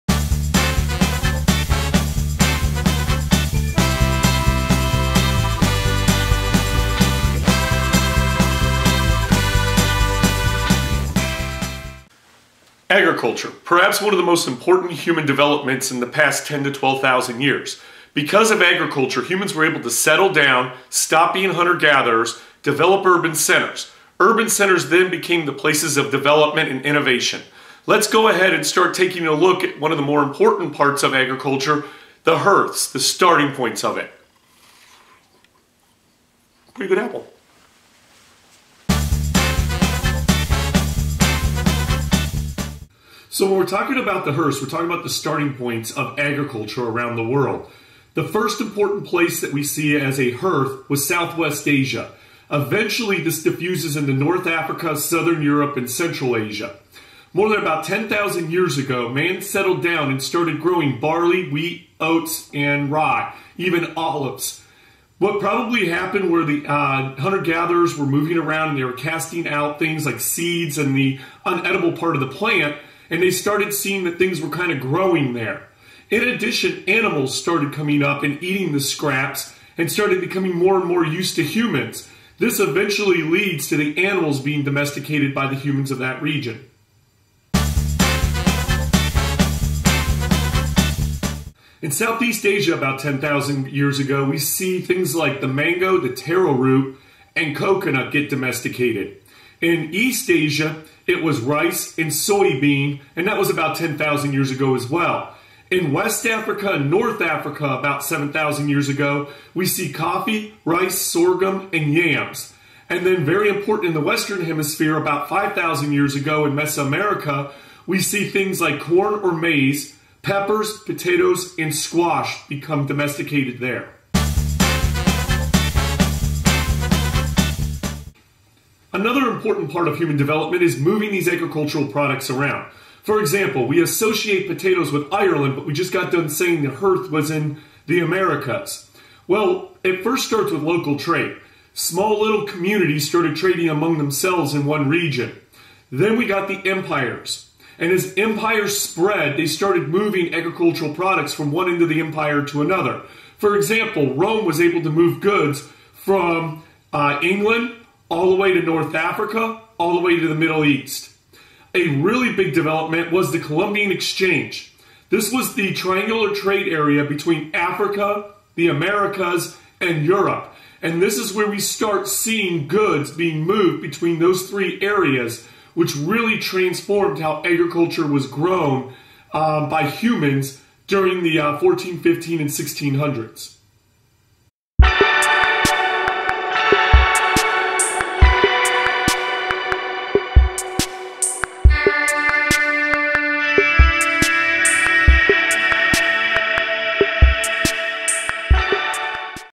Agricultural Hearths/Agriculture Part 1 (AP Human Geography)
A short video lesson on Agricultural Hearths for AP Human Geography.